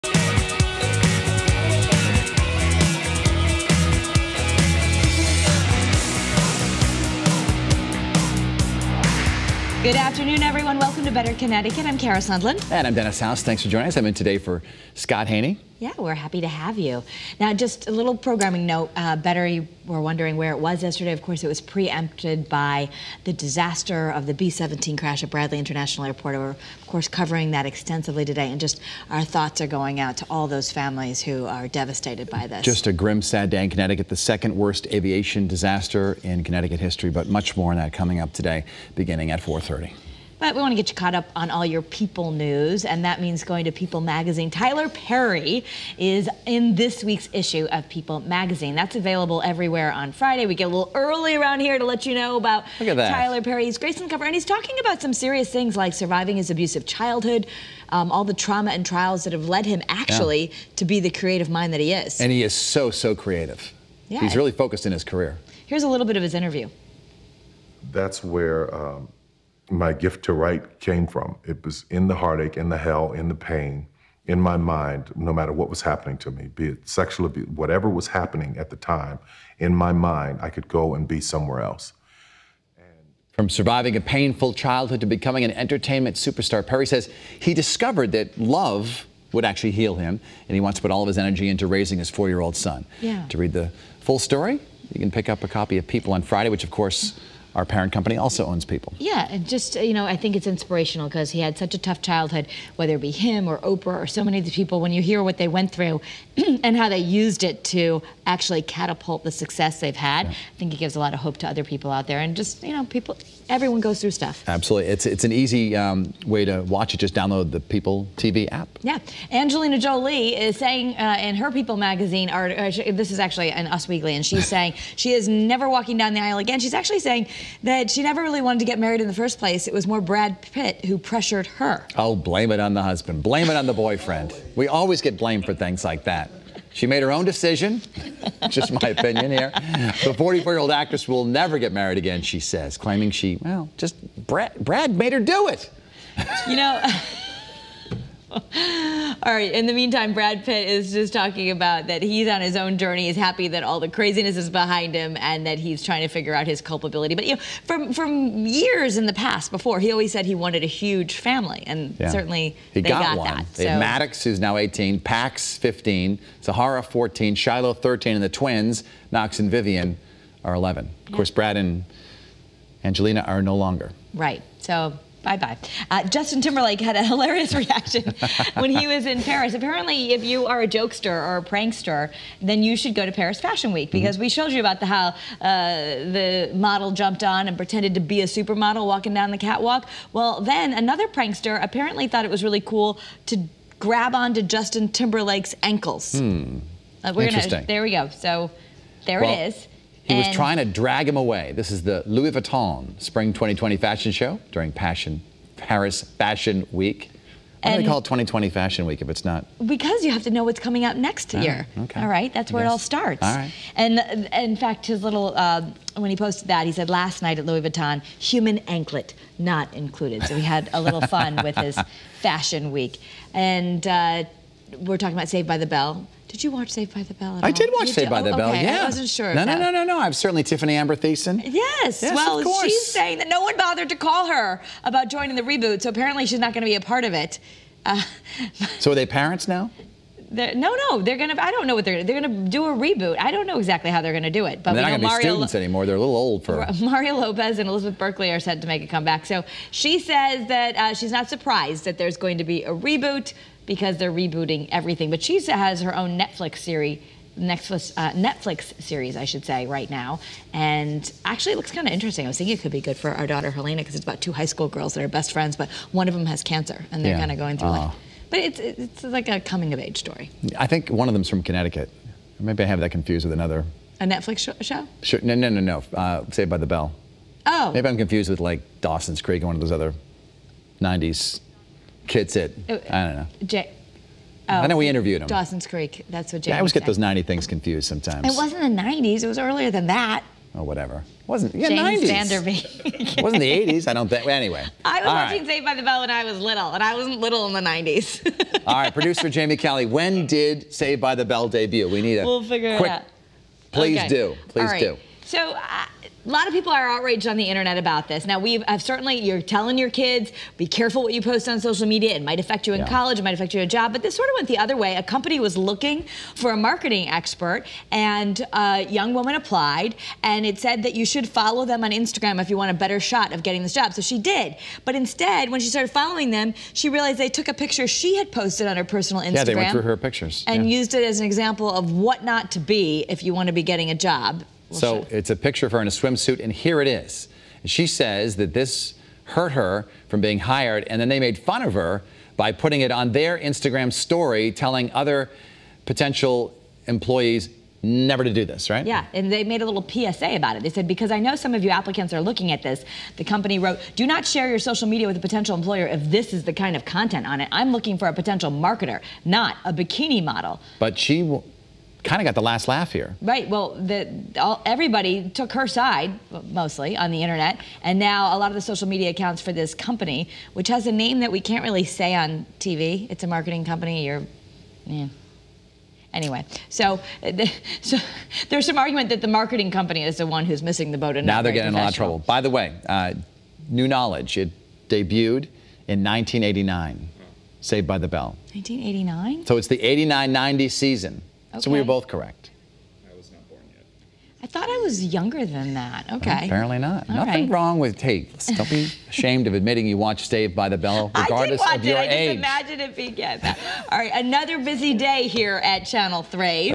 Tyler Perry on People Magazine, Angelina Jolie on marriage, and job applicant outrage
Kara and Dennis have today’s headlines